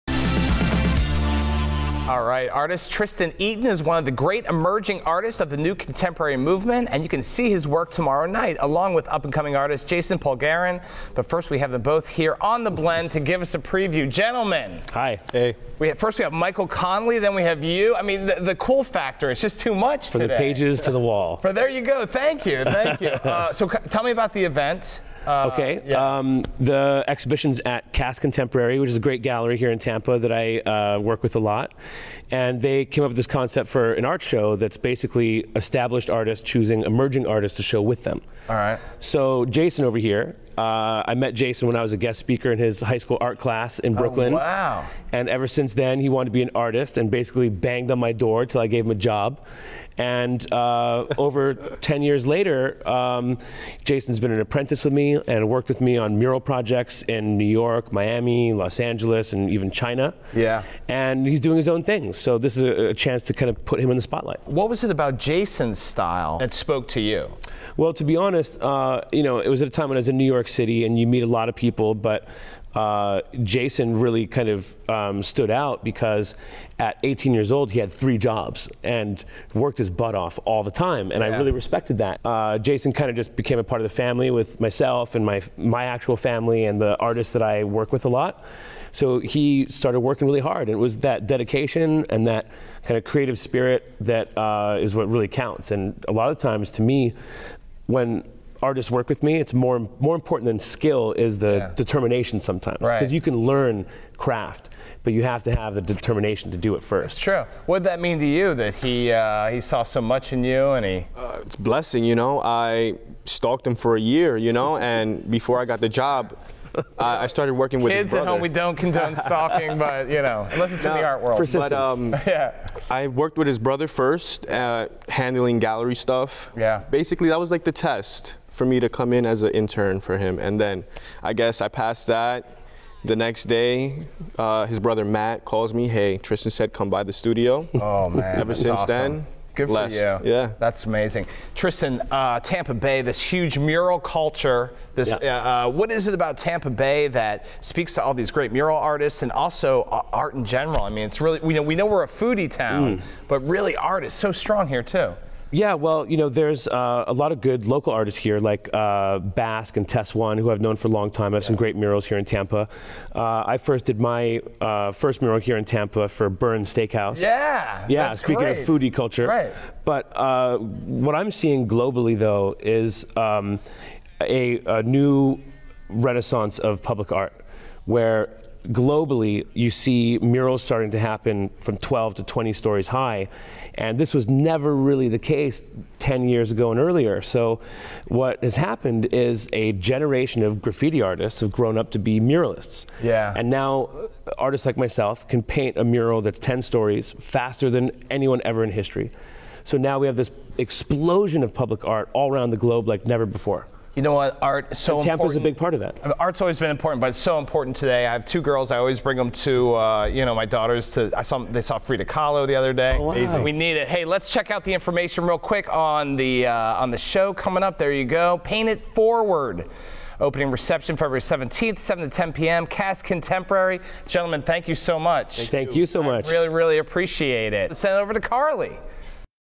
Tristan Eaton
We talk with the artist about his upcoming works.